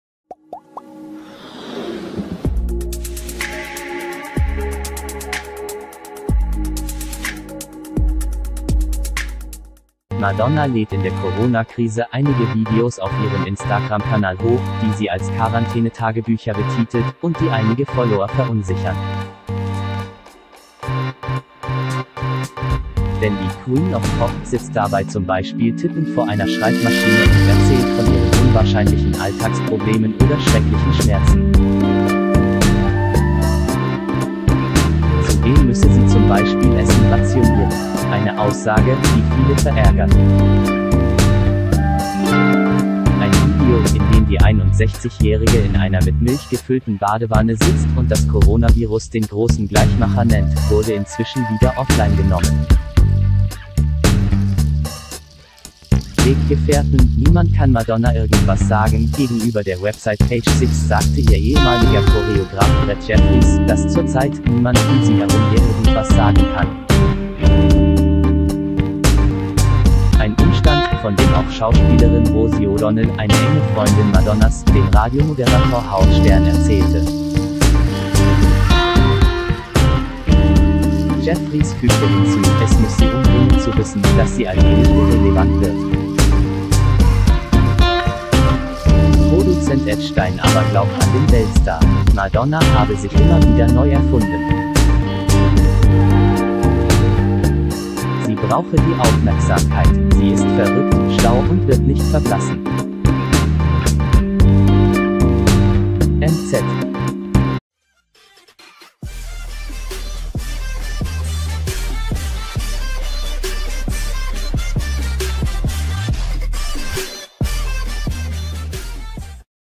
✅ Madonna lädt in der Corona-Krise einige Videos auf ihrem Instagram-Kanal hoch, die sie als Quaran
✍   Madonna lädt in der Corona-Krise einige Videos auf ihrem Instagram-Kanal hoch, die sie als Quarantäne-Tagebücher betitelt - und die einige Follower verunsichern. Denn die „Queen of Pop“ sitzt dabei zum Beispiel tippend vor einer Schreibmaschine und erzählt von ihren unwahrscheinlichen Alltagsproblemen oder schrecklichen Schmerzen. Zudem müsse sie zum Beispiel Essen rationieren. Eine Aussage, die viele verärgert.   Ein Video, in dem die 61-Jährige in einer mit Milch gefüllten Badewanne sitzt und das Coronavirus den großen Gleichmacher nennt, wurde inzwischen wieder offline genommen.   Weggefährten: „Niemand kann Madonna irgendwas sagen“ Gegenüber der Website Page Six sagte ihr ehemaliger Choreograph Brad Jeffries, dass zurzeit „niemand um sie herum ihr irgendwas sagen kann“. Ein Umstand, von dem auch Schauspielerin Rosie O'Donnell, eine enge Freundin Madonnas, dem Radiomoderator Howard Stern erzählte. Jeffries fügte hinzu: „Es muss sie umbringen zu wissen, dass sie allmählich irreleva ...